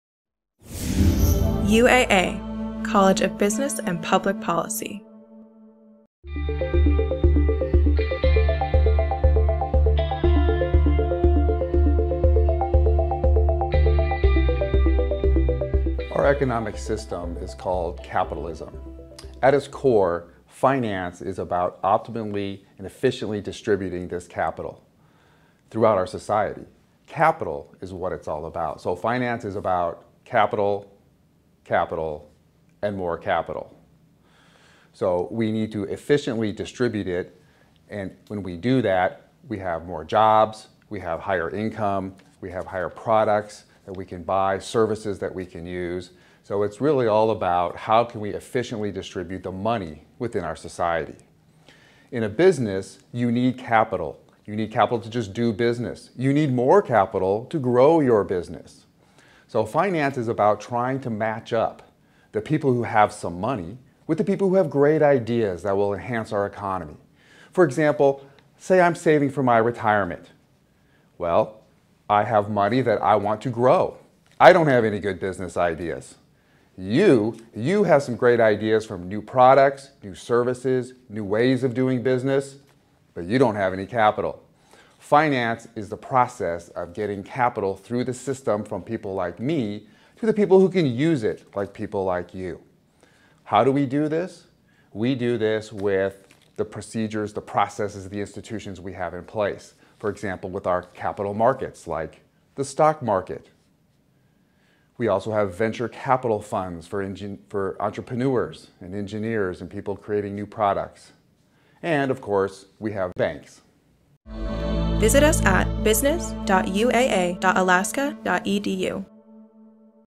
What is Finance About?
College of Business and Public Policy's Dr. John Nofsinger, William H. Seward Chair in International Finance, explains what finance is about.